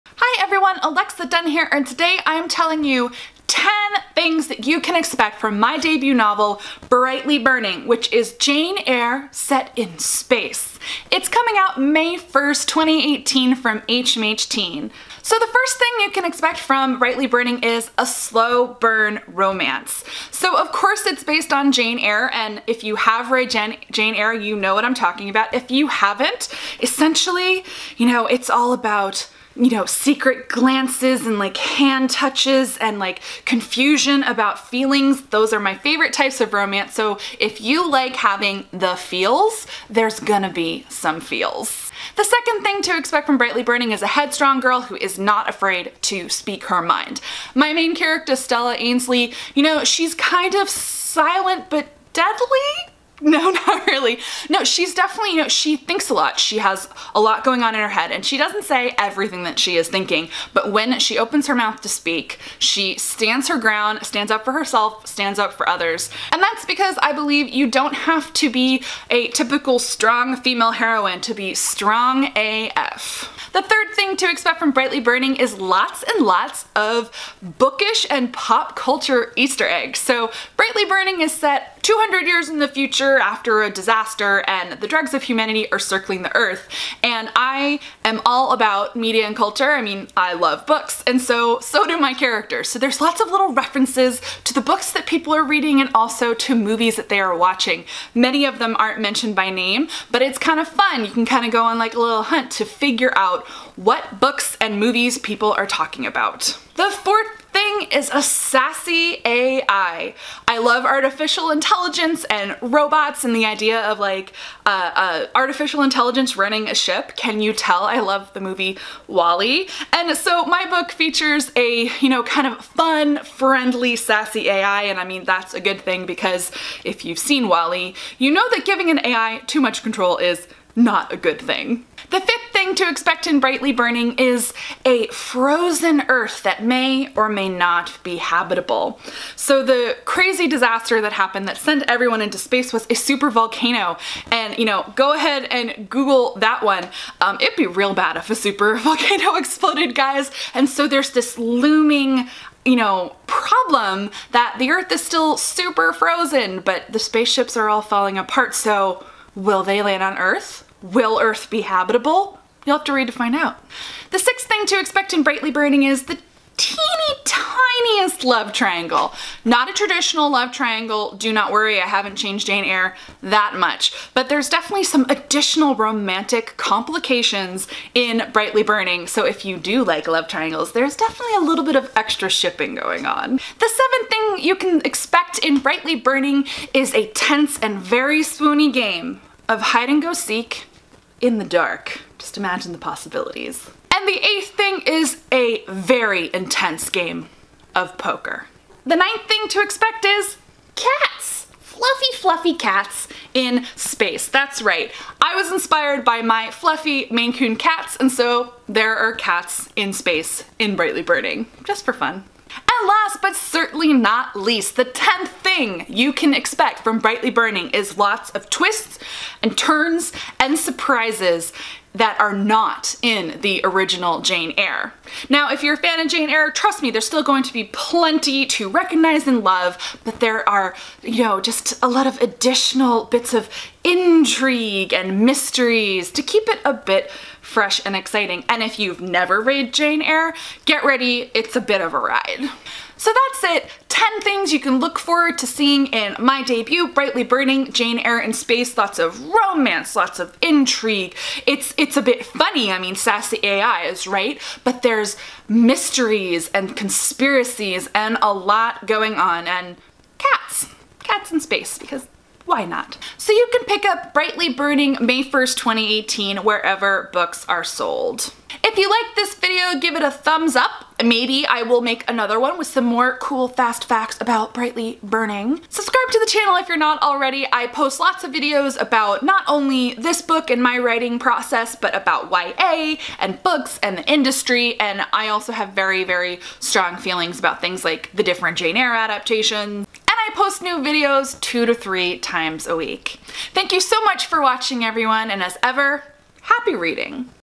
10 Things To Expect from Brightly Burning, aka Jane Eyre in SPACE!
What can you expect from my debut novel, Brightly Burning, which is coming out May 1, 2018 from HMH Teen? I'm coming at you with 10 fast facts/things to expect from the book! They range from the expected (romance!) to the not-so-expected (CATS!)... I hope your interest is piqued!

Pre-Order BRIGHTLY BURNING